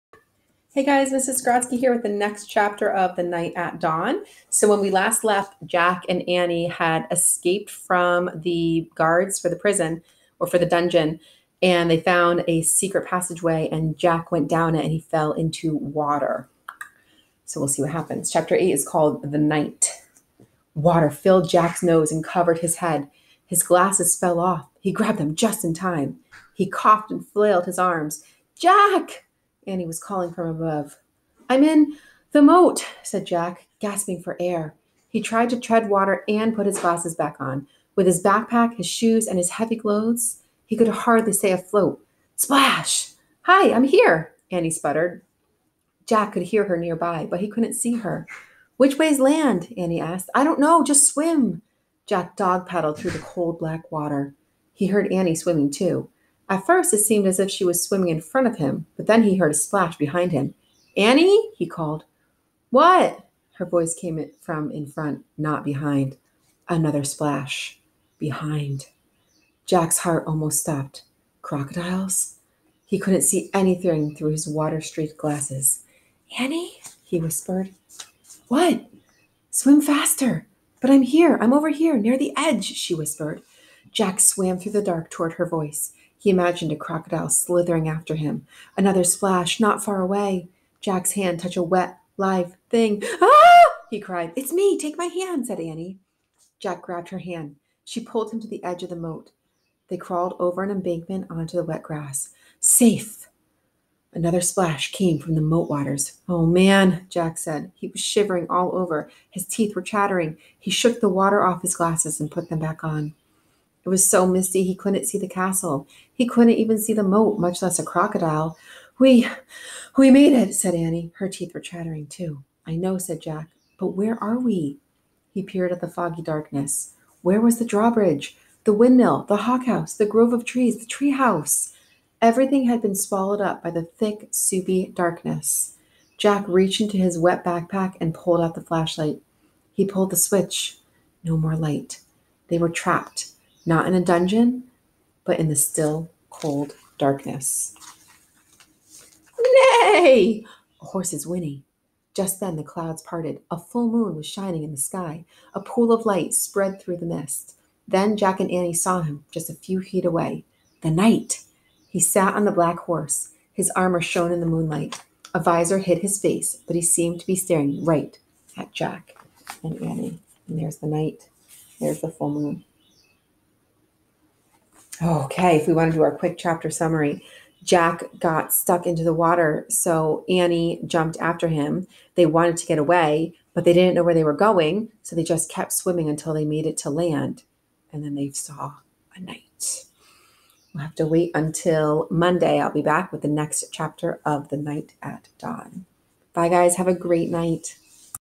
Chapter 8 of The Knight at Dawn
A chapter 8 read aloud of The Knight at Dawn by Mary Pope Osborne